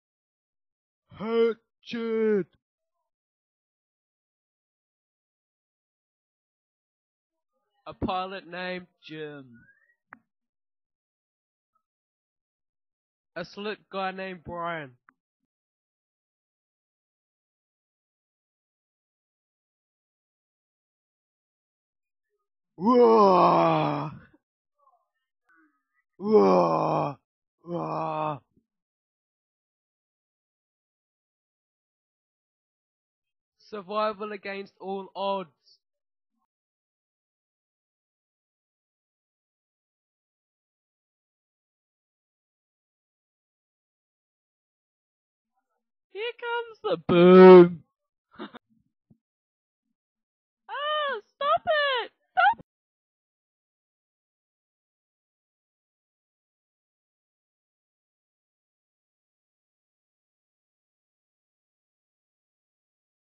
Hatchet film trailer 3: Jacob, Eli, Jordan, Shaden
A 9PJ English project at Kuranui College, Greytown, NZ - Imagine that 'Hatchet' the novel, written by Gary Paulsen has been turned into a film.  Make a promotional trailer for this film.

Made by Jacob, Eli, Jordan and Shaden